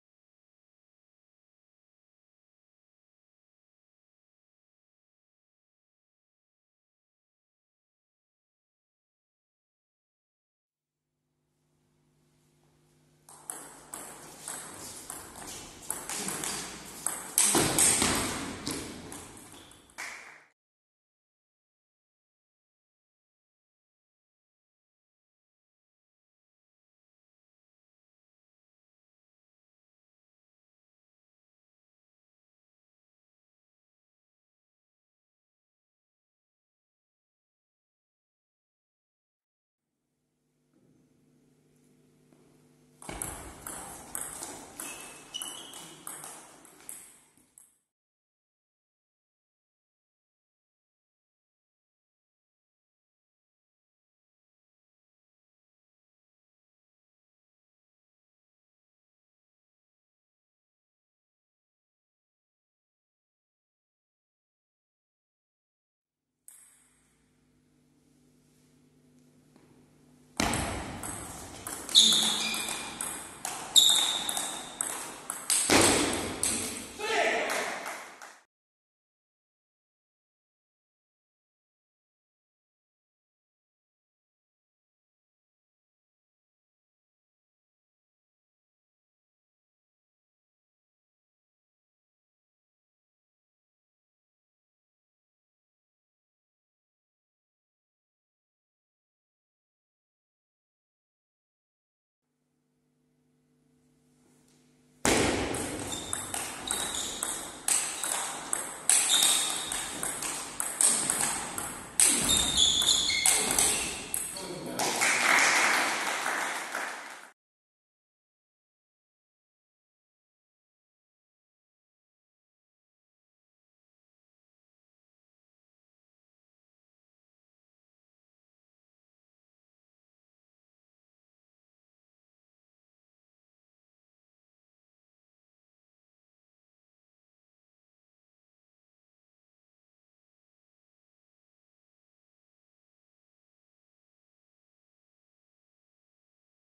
Joel vs Miguel - special moments
Joel Pinho vs Miguel Branco - special moments

Campeonato Nacional 2ª. Divisão ACR Saavedra Guedes vs AR Canidelo 'A' 4-1 6ª Jornada/1ª Volta - Zona Centro/Norte masculinos - Época 2018/2019 sábado, 30 de novembro - 16h00 Sala da ACR Saavedra Guedes - Rua Prof. Saavedra Guedes, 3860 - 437 Pardilhó Resultados e classificações + Info: Federação Portuguesa de Ténis de Mesa Ténis de Mesa na Associação Recreativa de Canidelo - Arc Ténis de Mesa